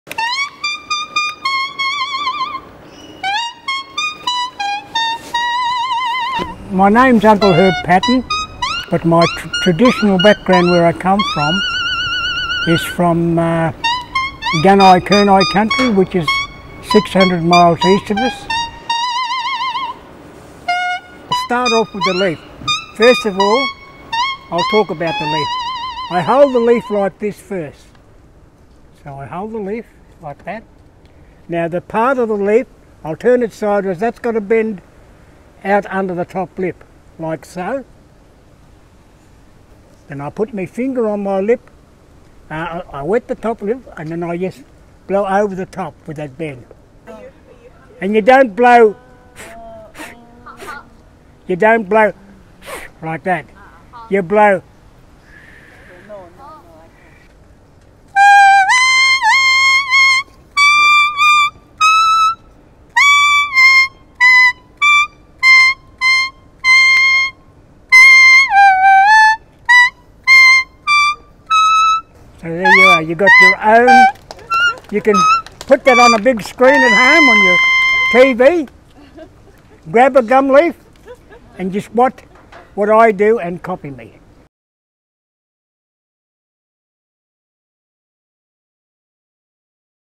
How to play music from a leaf
Aboriginal Australian, Uncle Herb teaches you how to play music out of a gum leaf